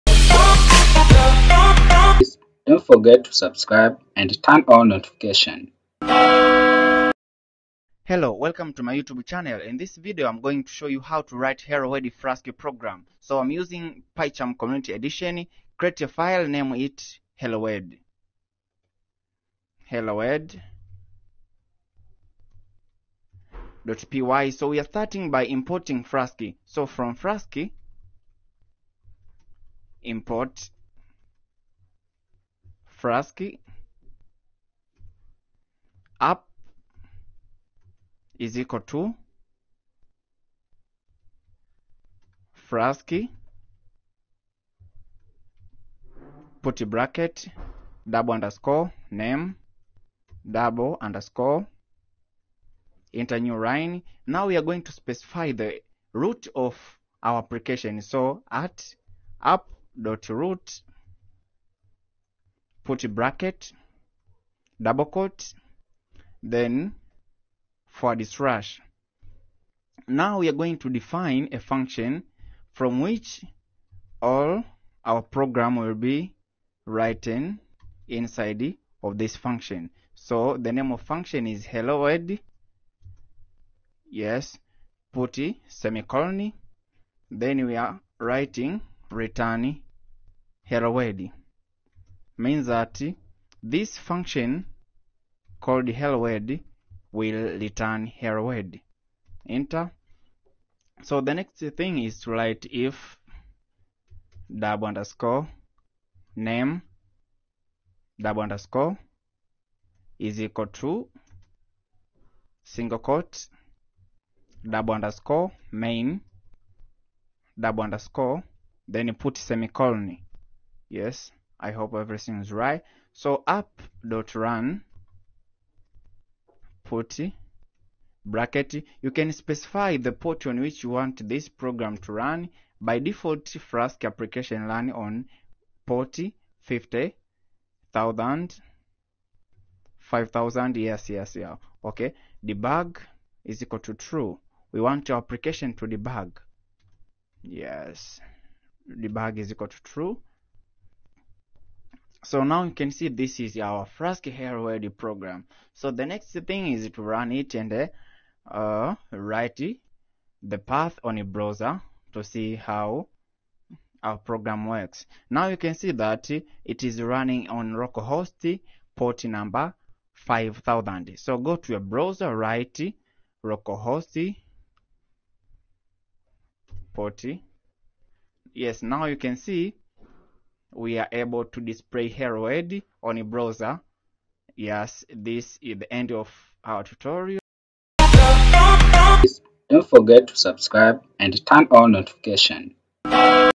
Python Flask : Hello World
Flask hello world program, in this video am going to show you how to write hello world program in Flask python web framework